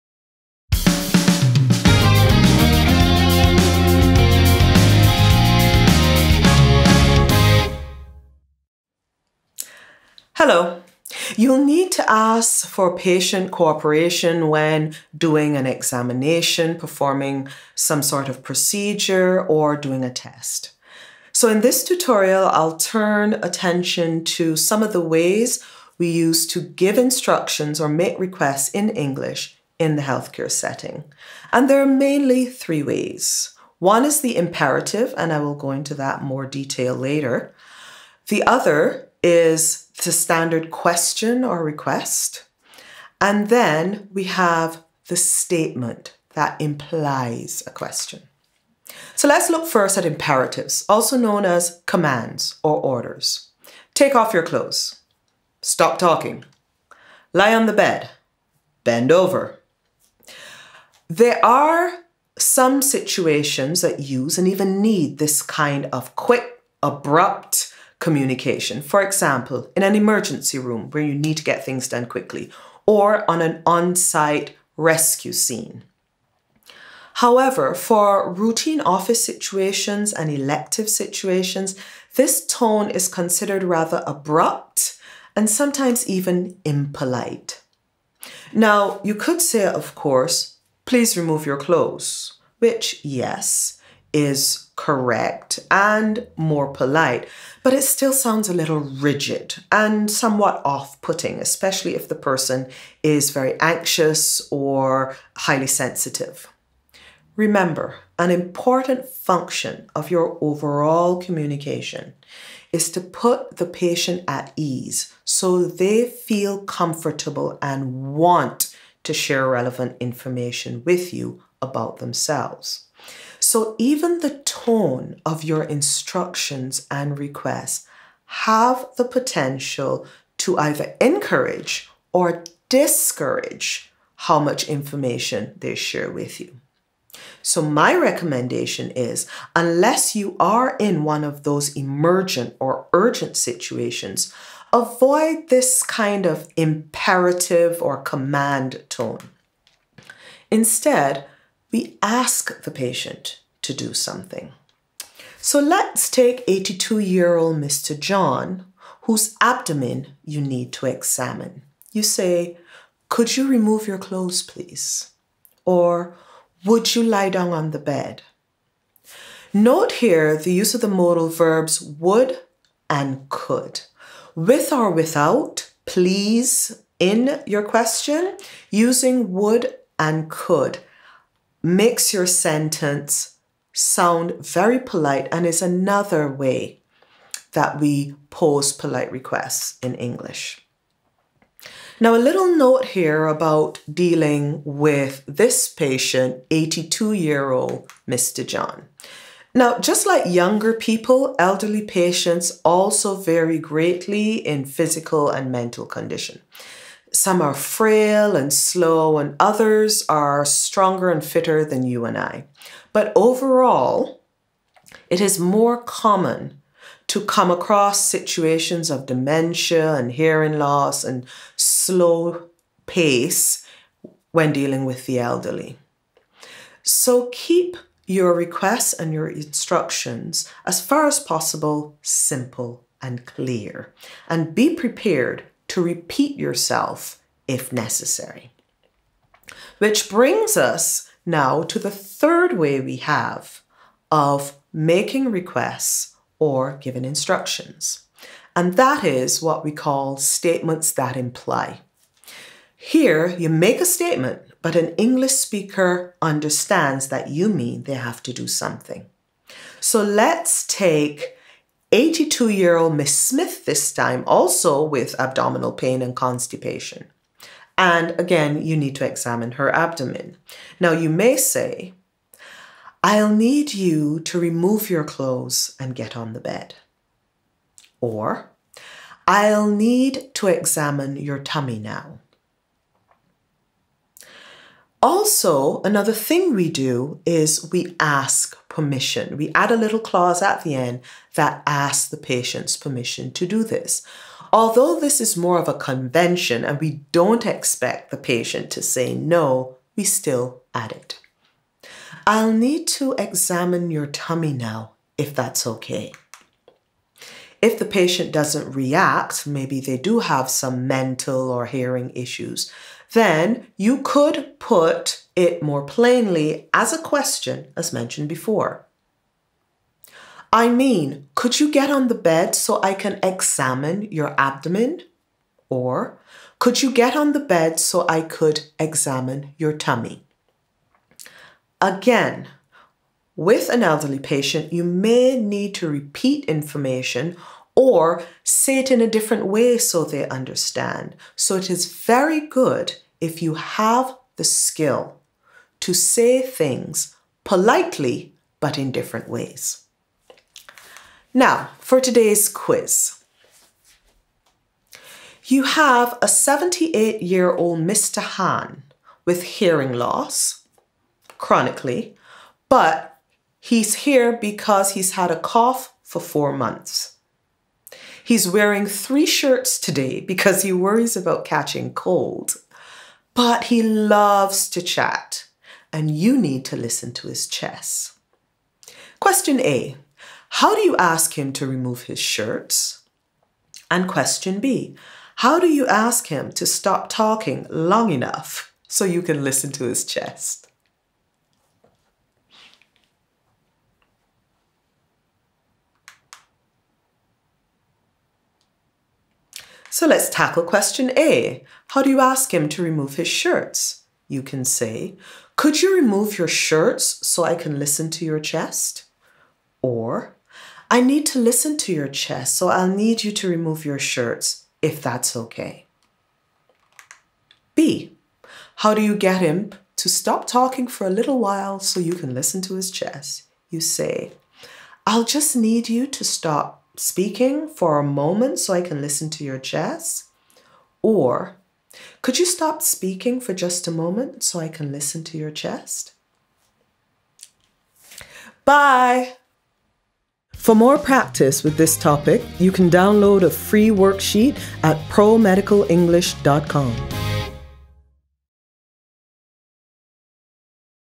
Quick OET Tutorial 16 + quiz - Language for the Elderly | Requests and Instructions
Join us in a Quick OET Tutorial session as we delve into the crucial topic of: Language for the Elderly | Requests and Instructions.
Improve vocabulary, grammar, speaking, writing, and listening skills, and more!

Do the right course with Banfield's Pro Medical English, pass OET, and get on with your future